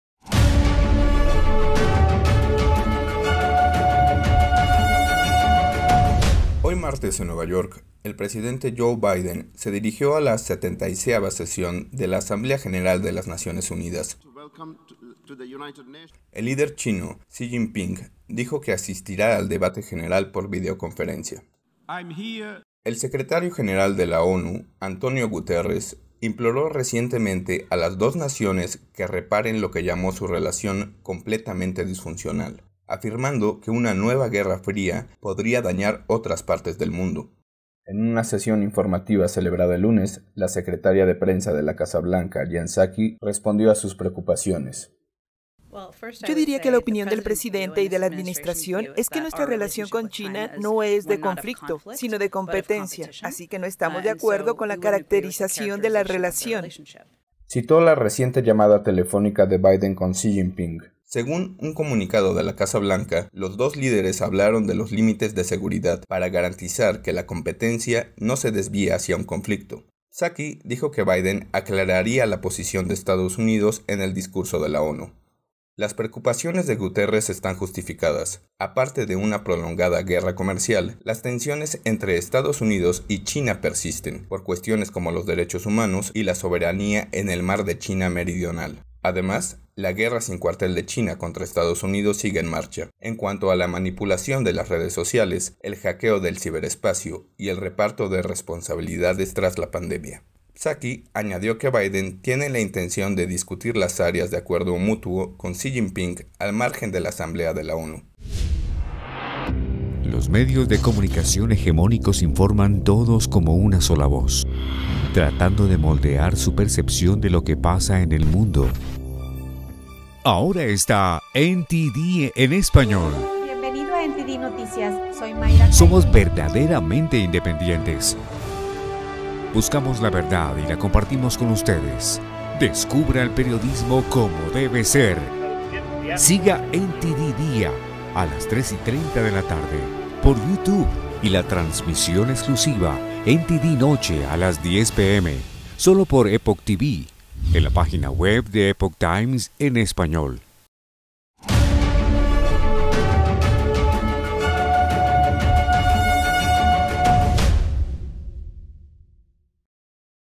La Casa Blanca niega una 'guerra fría' con China
Antes del discurso que Biden dio hoy en la Asamblea General de la ONU, la secretaria de prensa de la Casa Blanca, Jen Psaki, dijo que Estados Unidos no busca el conflicto con China. Eso después que el jefe de las Naciones Unidas planteara su preocupación por una posible guerra fría entre las dos grandes potencias.

💎  ¿Defenderá EE.UU. a Taiwán en caso de que estalle una guerra con China?